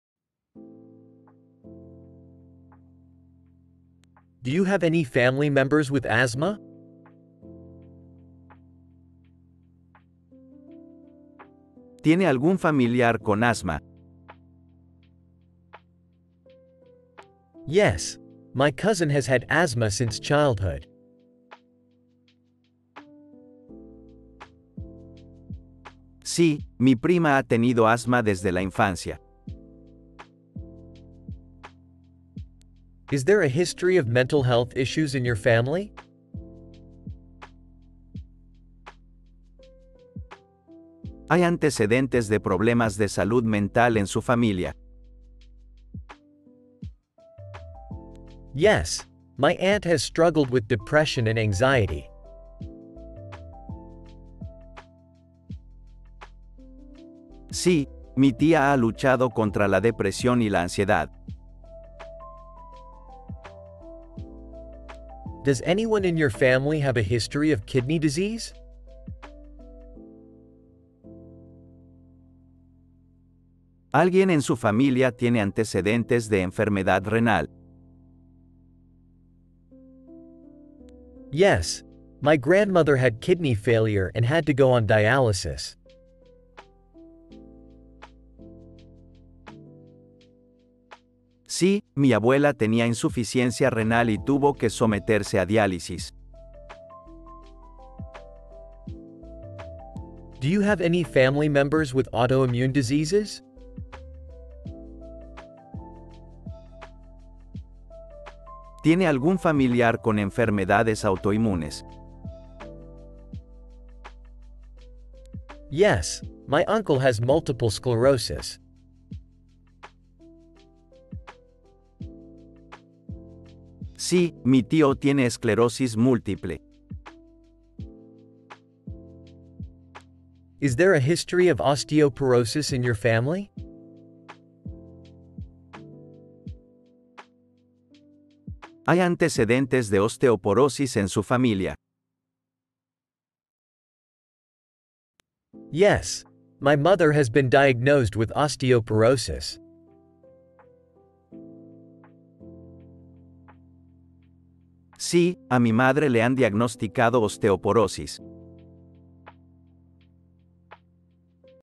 Family history dialogue (part 2) | Medical Spanish | Language Workouts
Practice your medical Spanish! Build your muscle memory by saying as many sentence reps out lout as possible.